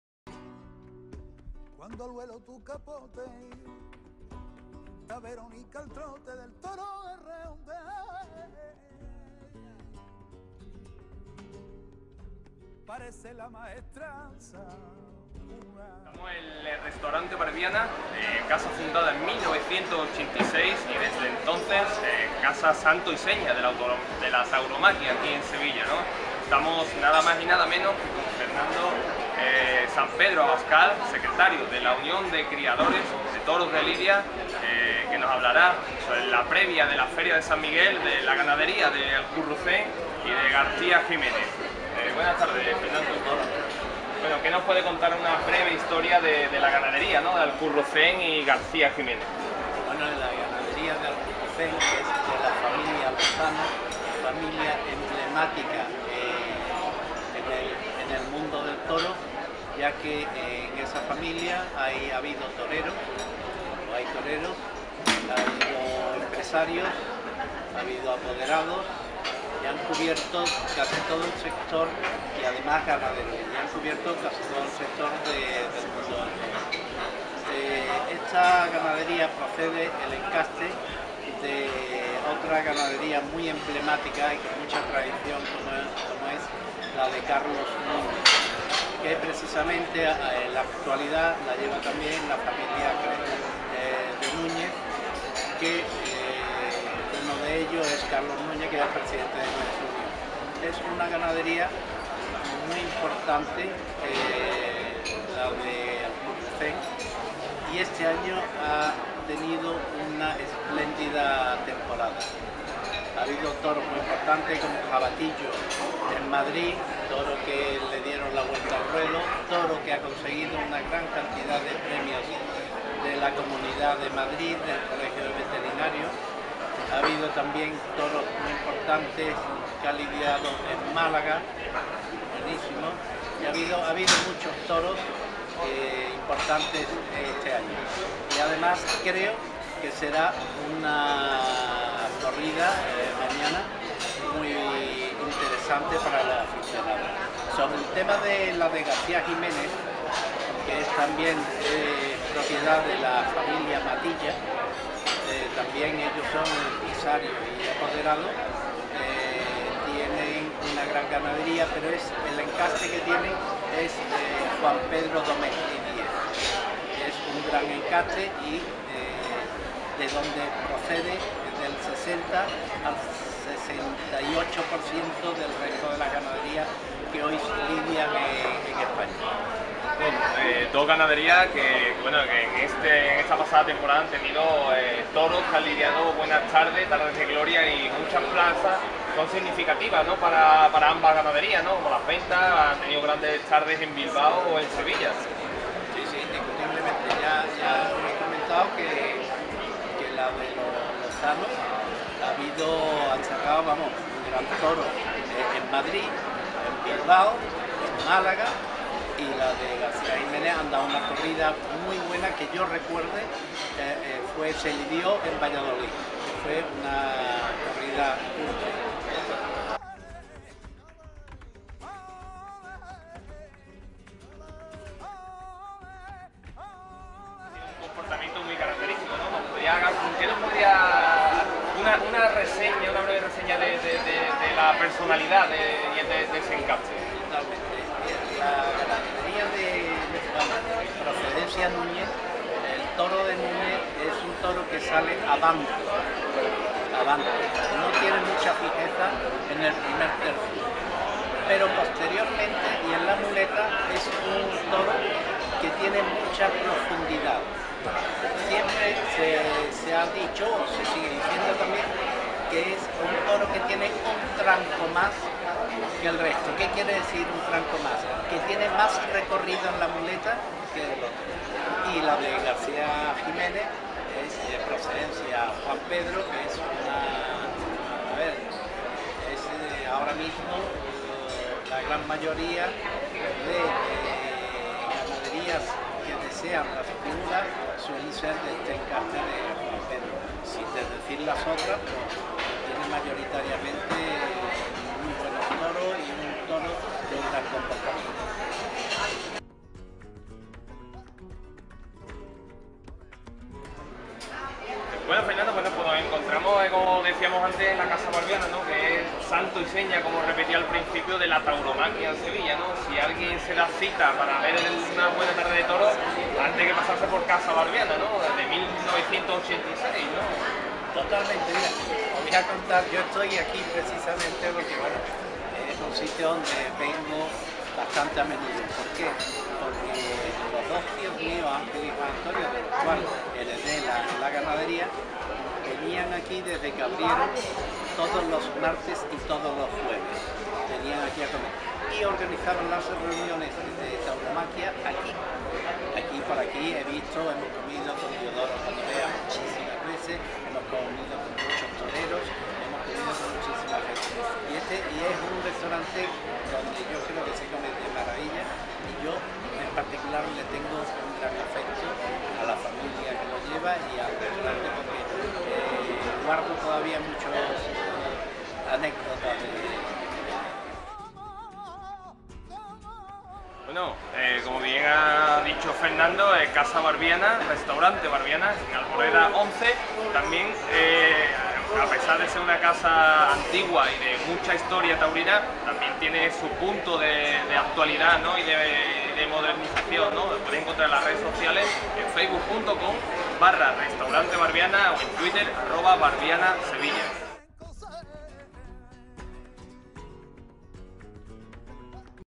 Entrevista con D. Fernando Sampedro Abascal en el Restaurante Barbiana
Entrevista desde el Restaurante Barbiana con D. Fernando Sampedro Abascal, secretario de la Unión de Criadores de Toros de Lidia, quién nos hablará sobre las ganaderías de Alcurrucén y García Jiménez. Ambas ganaderías estarán presente en la Plaza de Toros de la Real Maestranza en la feria de San Miguel 2016.